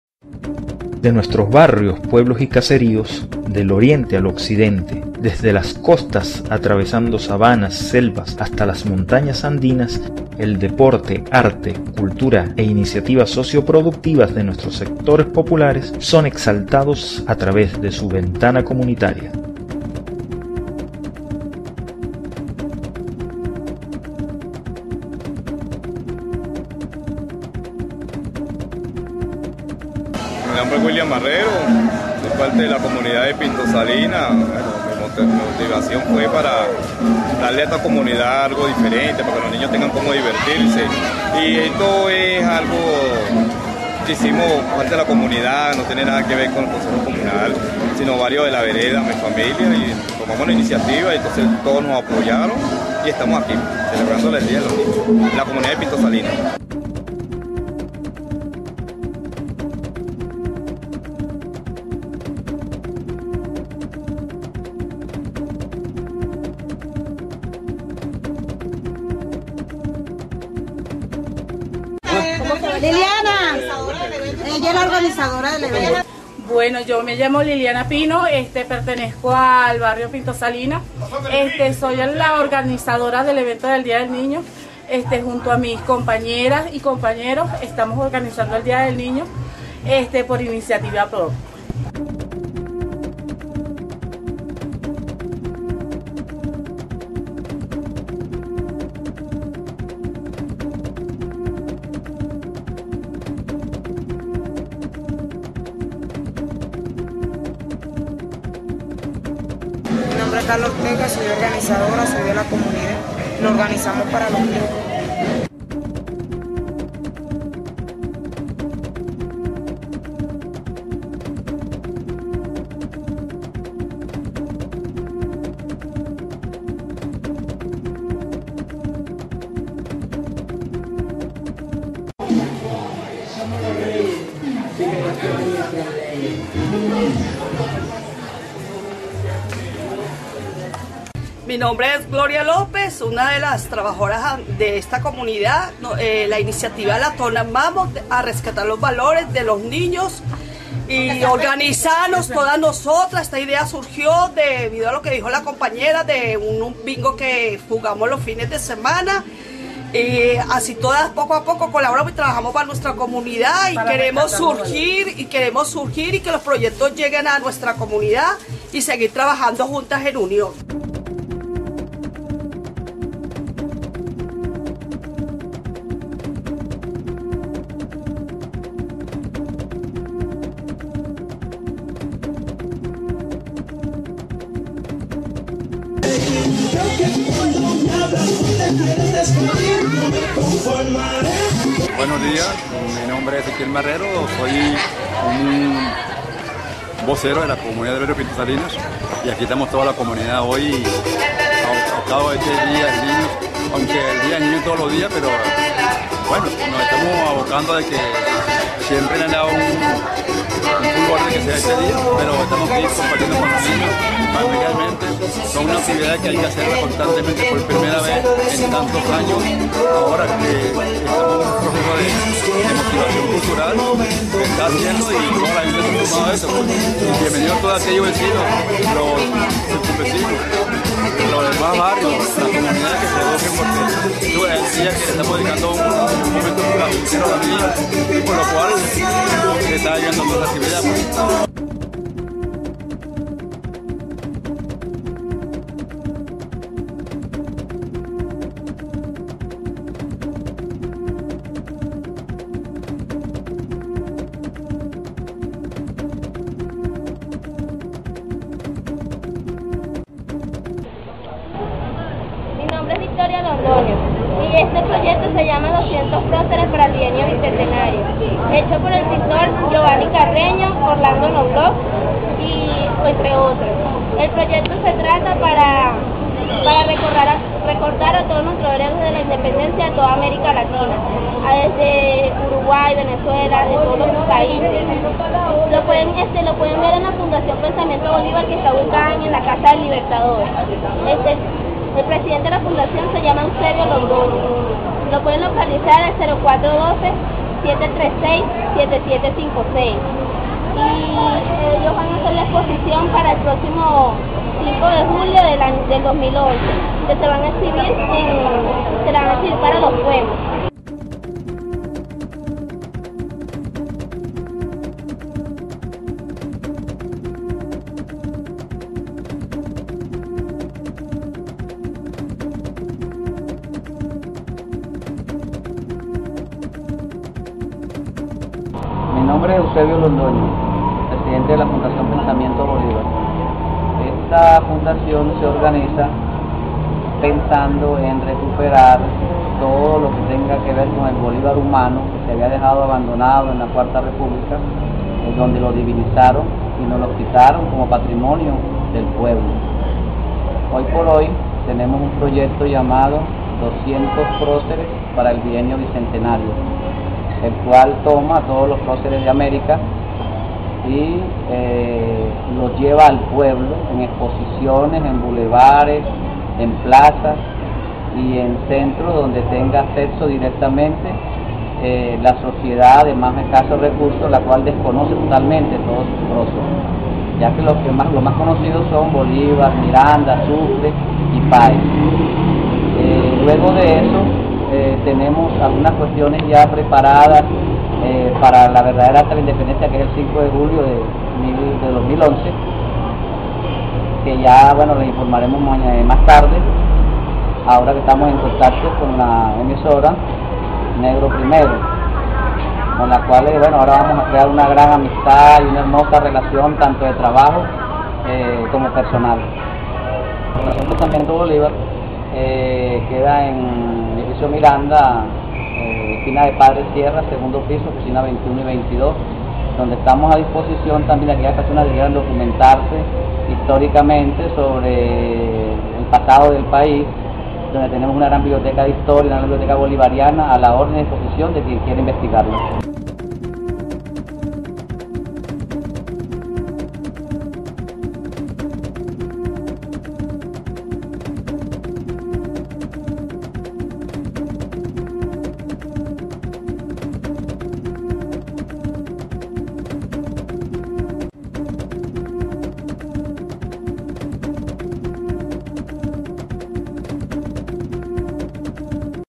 Ventana comunitaria 12
Vamos a conocer el esfuerzo de vecinos de Pinto Salinas para formar a sus niños y sus niñas. Tambièn averiguaremos sobre el trabajo de la Fundaciòn Pensamiento Bolìvar